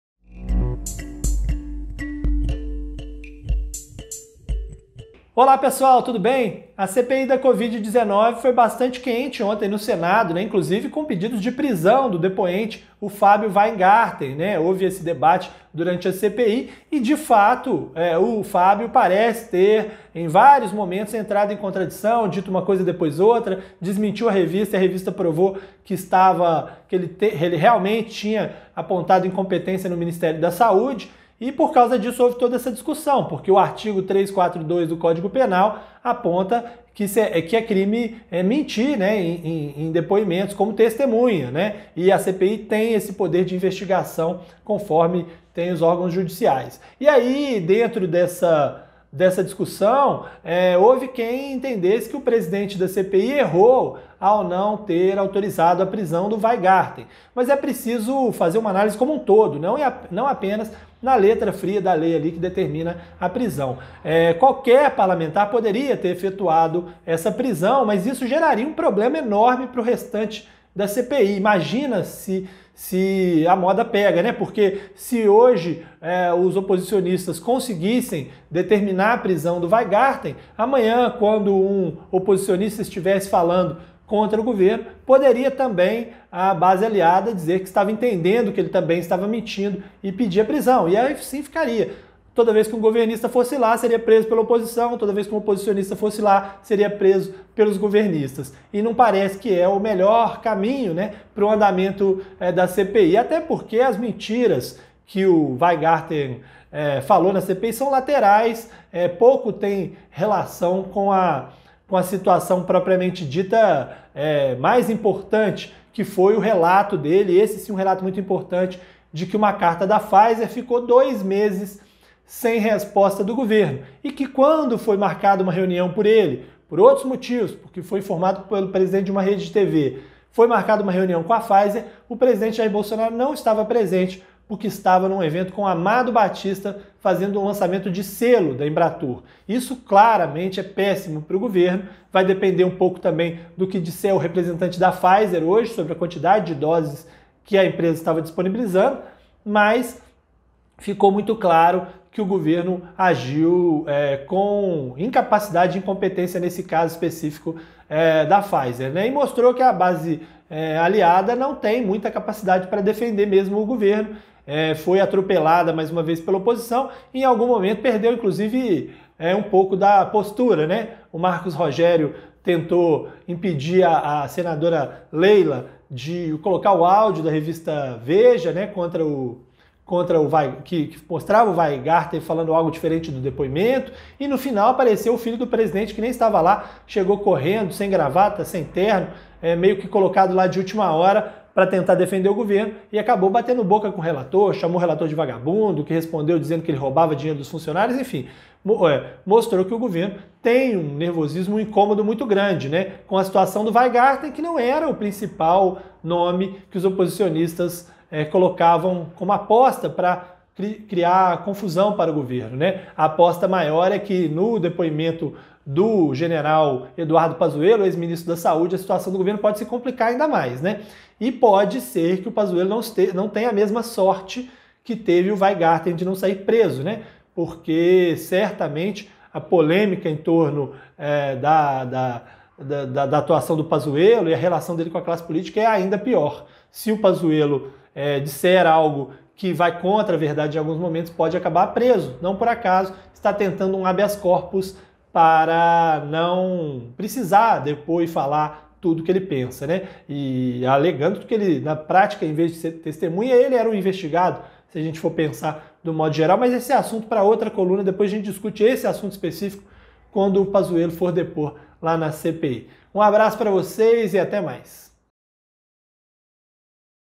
Prender ou não prender
Na coluna Política em Análise, o editor de O Tempo, Ricardo Corrêa, avalia a decisão do presidente da CPI da Covid, Omar Aziz, de não prender Fabio Wajngarten por falso testemunho após pedido do relator da Comissão, senador Renan Calheiros, e de outros parlamentares.